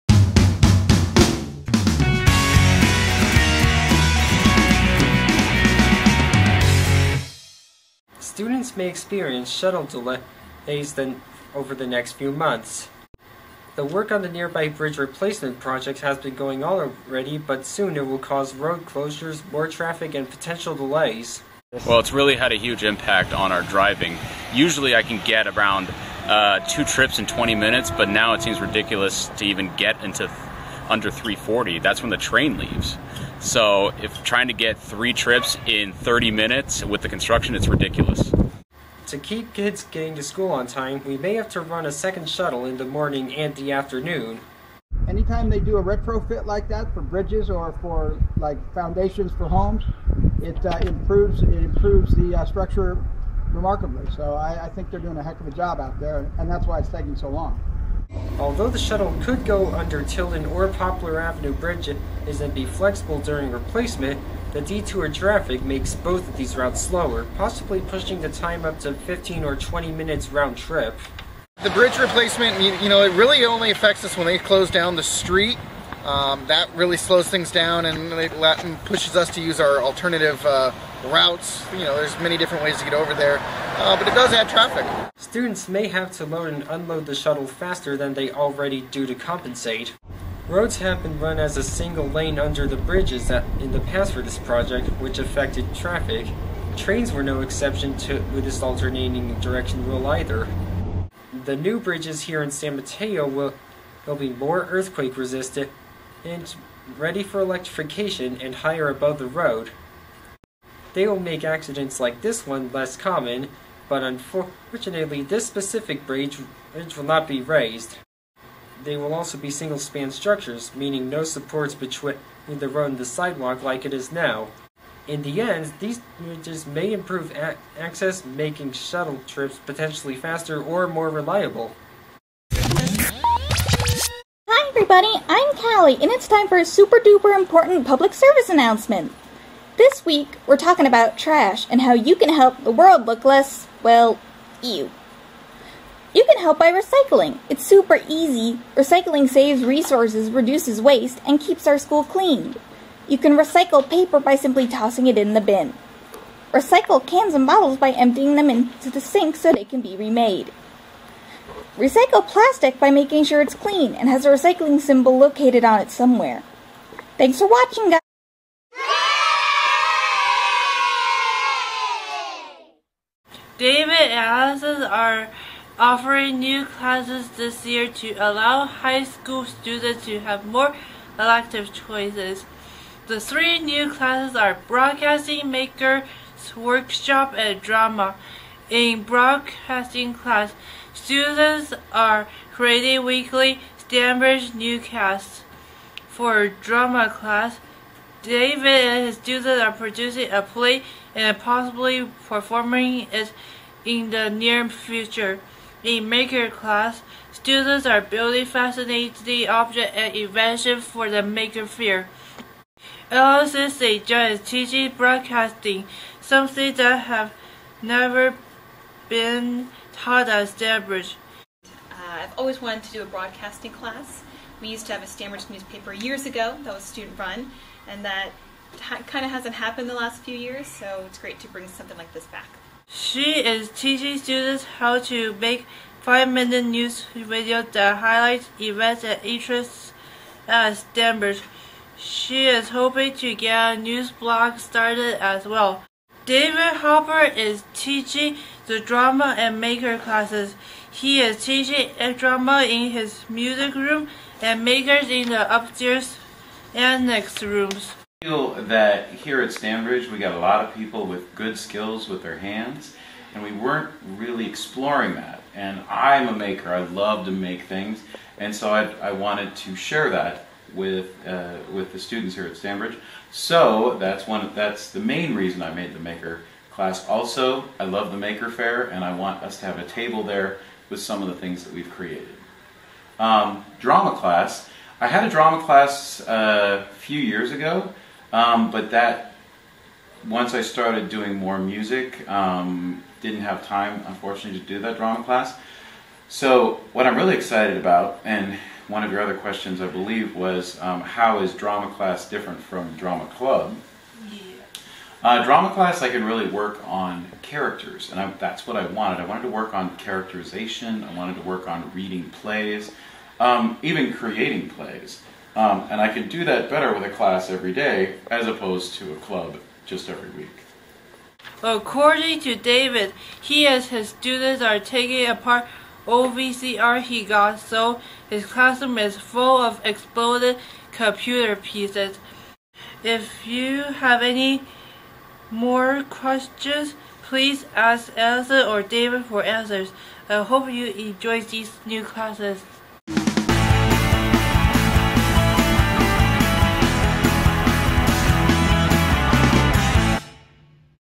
Student News Episode 1
Episode 1 (week of 9/21): On the premiere episode of Stanbridge Academy Student News, Daniel discusses the potential shuttle delays due to Caltrain construction on bridges in San Mateo (featuring interviews with Matt, James, and John B.), Kali the dog reports on the importance of recycling, and Arlene interviews Allison and David to learn more about their new high school electives.